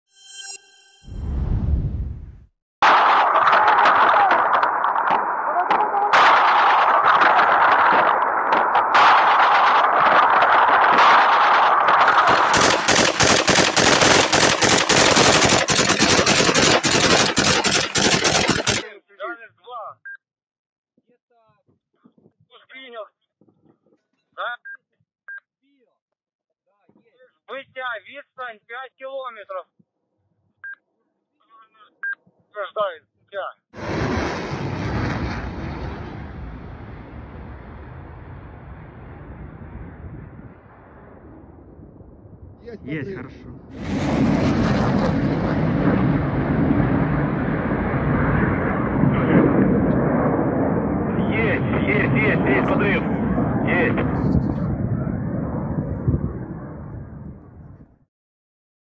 Повітряні сили показали відео бойової роботи проти шахедів у новорічну ніч
"Більшість «шахедів» цієї ночі було збито в зоні відповідальності повітряного командування «Південь».

На відео – бойова робота підрозділів зенітних ракетних військ та мобільних вогневих груп!
Дякую воїни!"

Джерело: Офіційний Телеграм-канал командувача Повітряних Сил Збройних Сил України генерал-лейтенанта Миколи Олещука